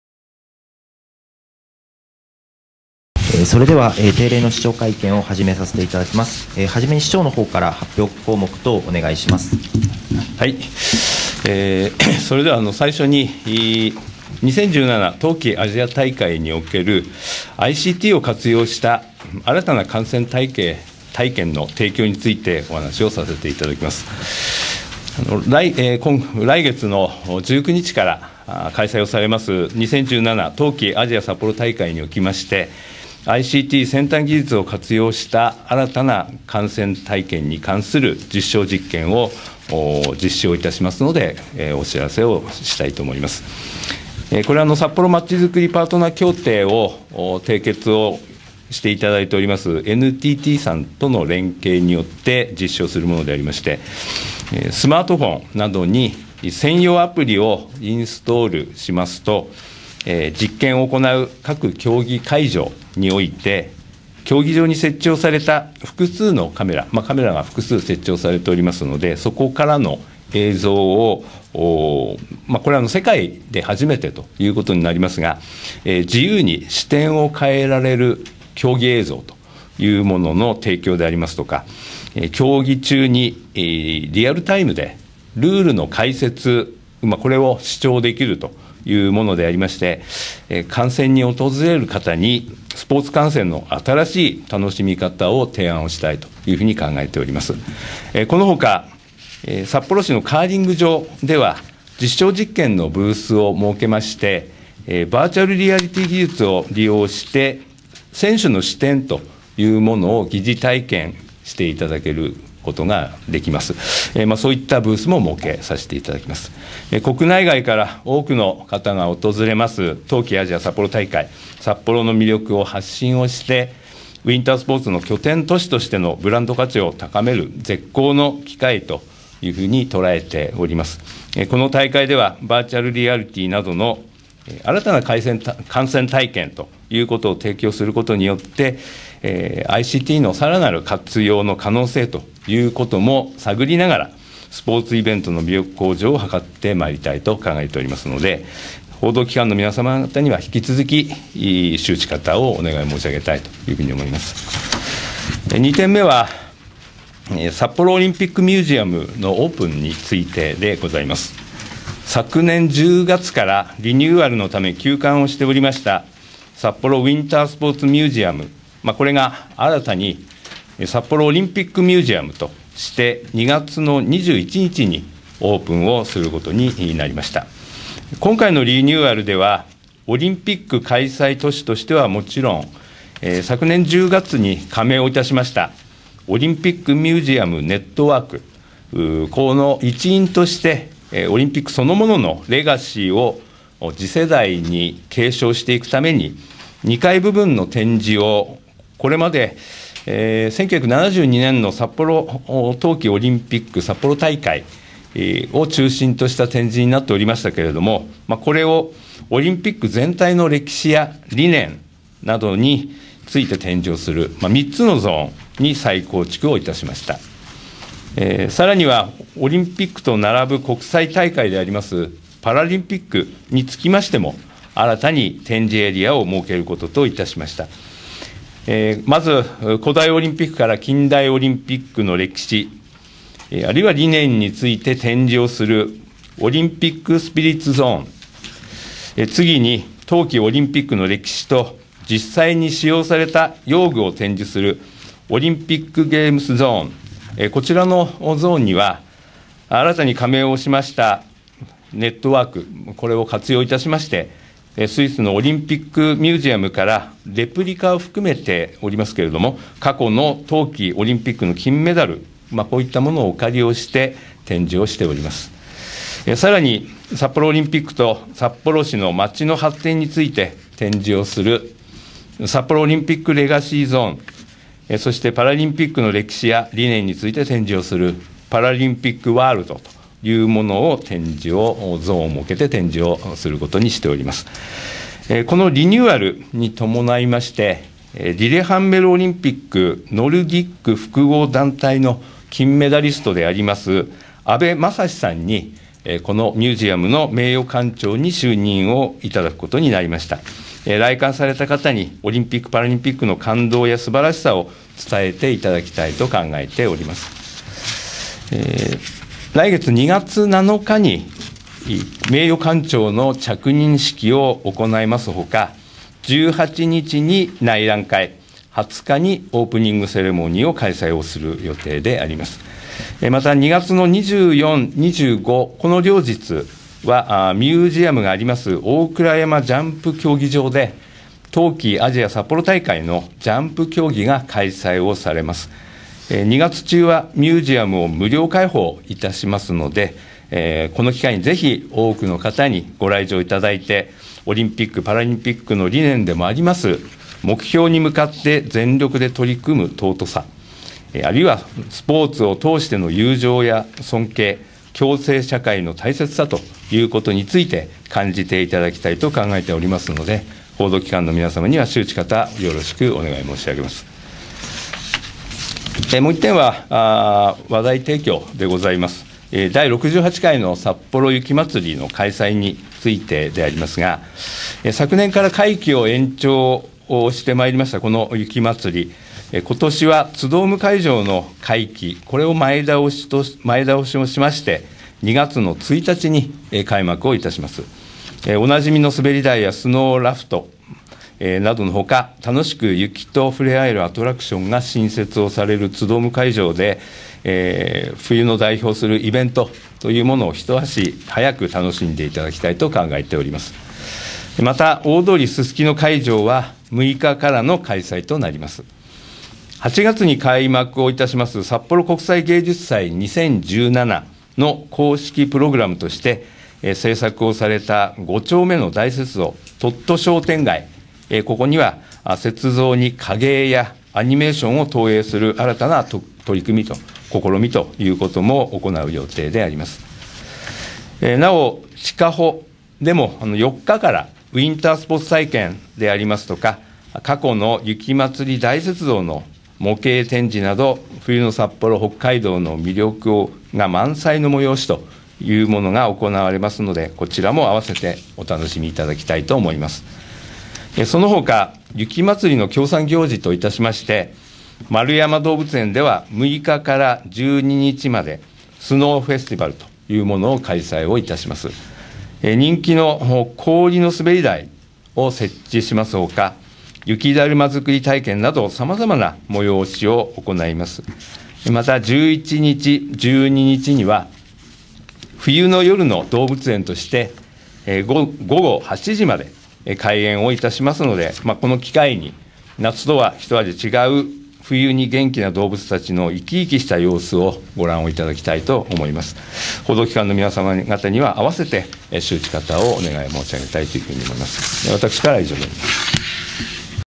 平成28年度第17回定例市長記者会見発表項目編
平成29年1月24日定例市長記者会見発表項目編です。項目は以下の通り。
・「2017冬季アジア札幌大会」におけるICTを活用した新たな観戦体験の提供について
・「札幌オリンピックミュージアム」のオープンについて
・「第68回さっぽろ雪まつり」の開催について